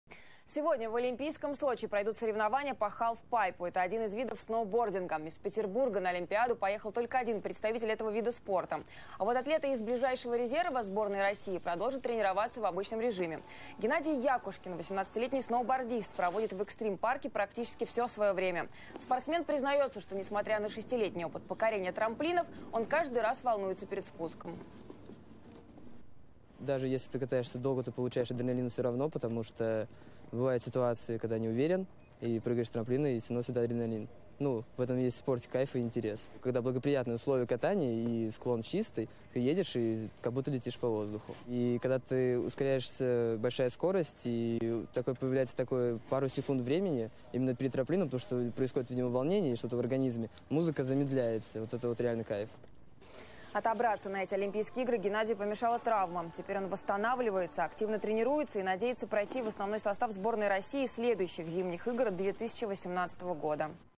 Петербуржец сноубордист выступит на Олимпиаде в Сочи
Сегодня в олимпийском Сочи пройдут соревнования по халф-пайпу - это один из видов сноубординга. Из Петербурга на Олимпиаду поехал только один представитель этого вида спорта. А вот атлеты из ближайшего резерва сборной России продолжают тренироваться в обычном режиме. Геннадий Якушкин, 18-летний сноубордист, проводит в экстрим-парке практически все свое время. Спортсмен признается, что, несмотря на 6-летний опыт покорения трамплинов, он каждый раз волнуется перед спуском.